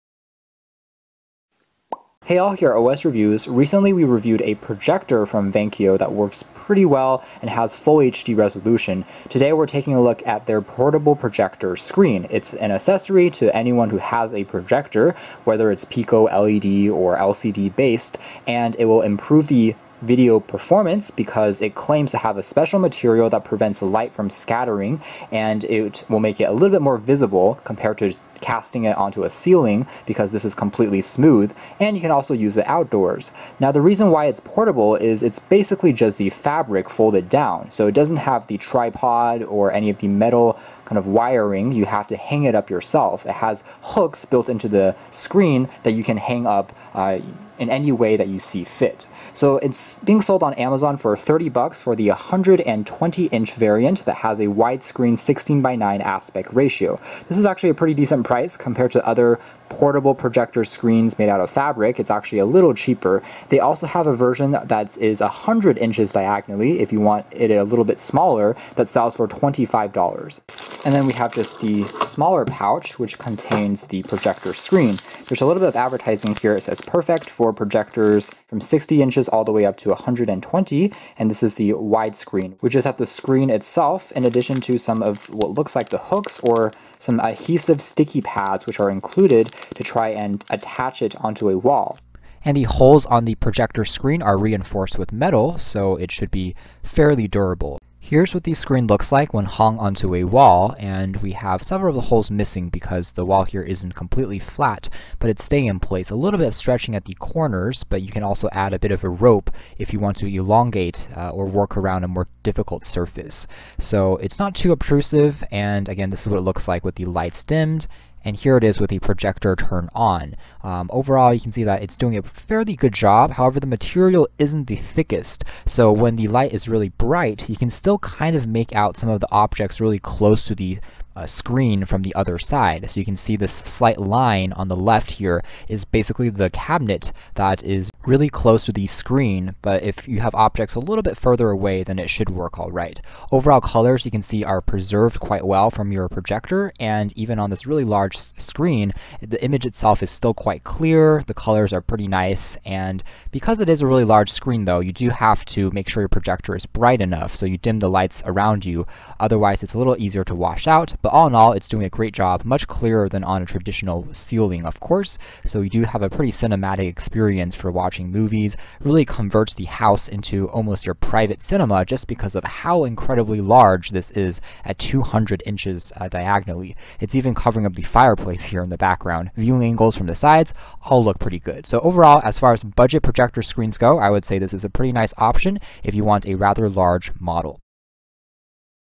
REVIEW: Vankyo 120" Portable Projector Screen (Widescreen)
VANKYO Projector Screen 120 Inches 16:9 Portable Projection

Details:
SPECIAL MATERIAL: Use special photosensitive material, effectively prevent light scattering, visible angle is wide.
FOLDABLE&PORTABLE: It is lightweight and can be folded with a projector screen is the most compact and travel-friendly option for home & outdoor cinema and office presentations
EASY-INSTALL: This unique projector screen with peel-and-stick hooks allows you to set up quickly and easily on any tabletop.
CREASELESS: Be made of flexible polyester material, this projector screen is more smooth and creaseless

Package:
1 x 120" Projector Screen
16 x Stickers
16 x Hooks